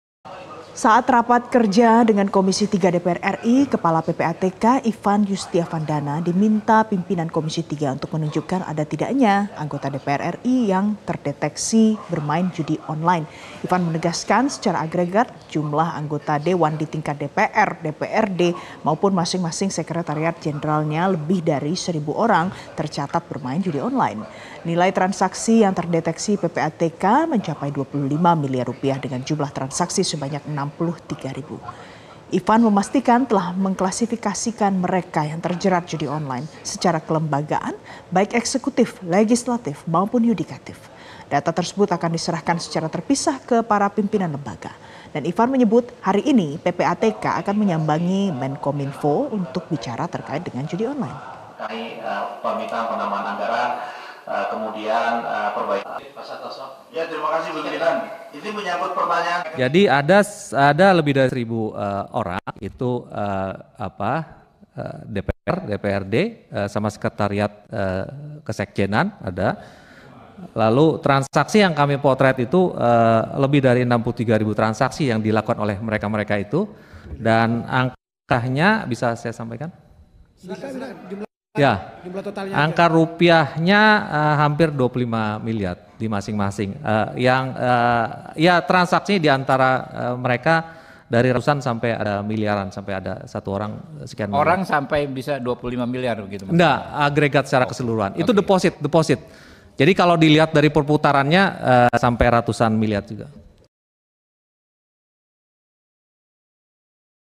PPATK KANTONGI NAMA DAN ALAMAT ANGGOTA DEWAN YANG MAIN JUDI ONLINE
Ribuan anggota Dewan Perwakilan Rakyat (DPR), Dewan Perwakilan Rakyat Daerah (DPRD), serta pegawai Sekretariat Jenderal DPR/DPRD terdeteksi main judi online. Pusat Pelaporan dan Analisis Transaksi Keuangan (PPATK) pun mengaku sudah mengantongi data lengkap para wakil rakyat yang memainkan aktivitas ilegal tersebut.